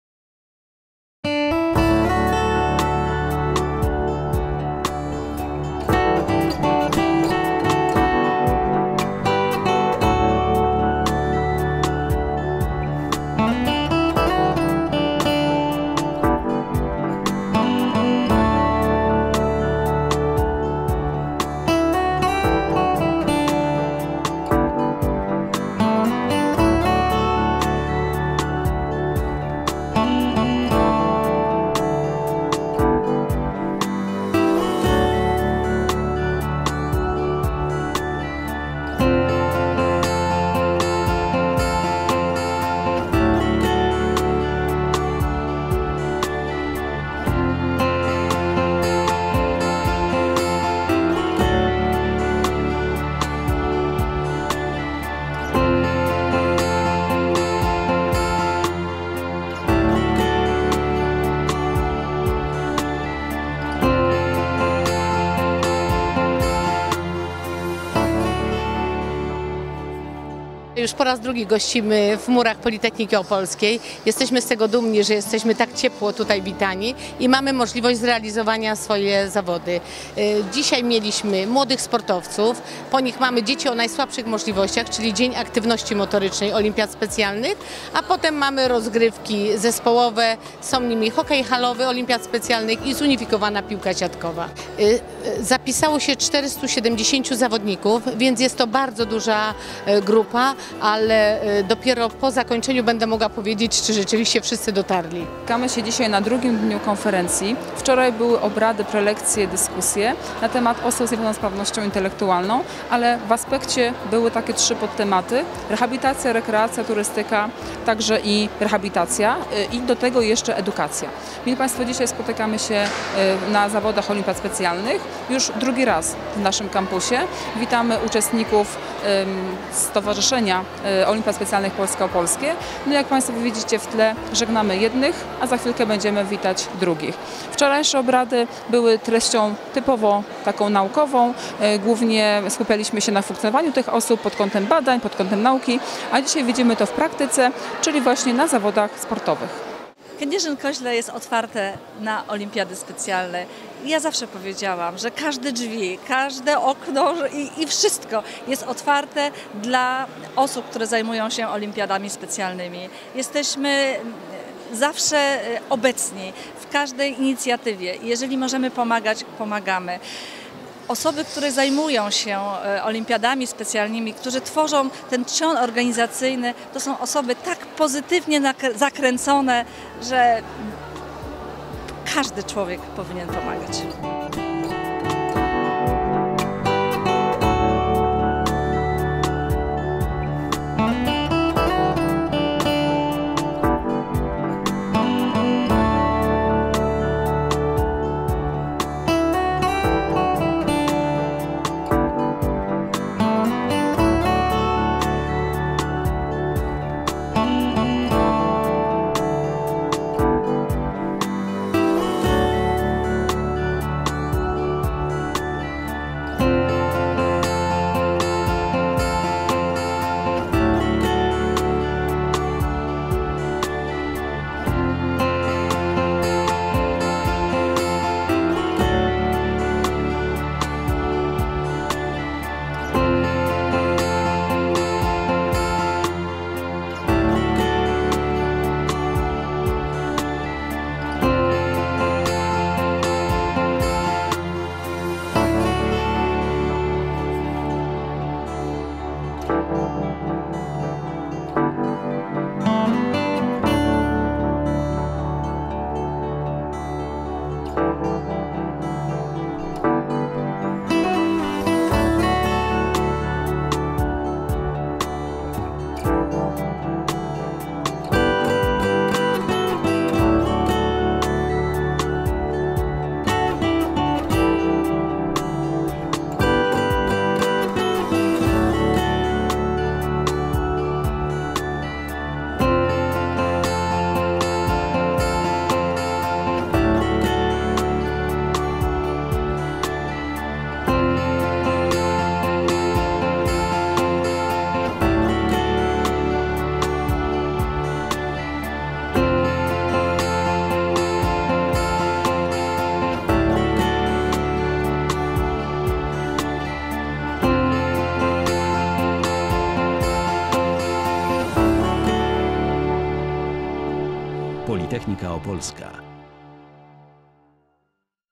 Druga Olimpiada Specjalna na Politechnice Opolskiej
W ramach warsztatów dla zawodników Olimpiad Specjalnych Polska zorganizowano Wioskę Olimpijską, Program Zdrowotny, w której mogli skorzystać z bogatego programu po ukończeniu zawodów.

Zawody składały się z trzech części; pierwsza Program „Młodzi Sportowcy” obejmuje dzieci do lat 8, druga próby sprawnościowe MATP (Program Treningu Aktywności Motorycznej), trzecią częścią był turniej piłki siatkowej zunifikowanej i hokeja halowego.

W trakcie zmagań sportowych, zawodnikom pomagali wolontariusze – studenci Wydziału Wychowania Fizycznego i Fizjoterapii Politechniki Opolskiej udowadniając, że młodzi ludzie chętnie pomagają i uczą innych funkcjonowania w społeczeństwie, otwierają się na edukację i inne potrzeby osób z niepełnosprawnościami. Z drugiej strony osoby potrzebujące pomocy utwierdzają pełnosprawnych w przekonaniu, że są wartościowymi, wrażliwymi ludźmi, pełni chęci do życia i zabawy...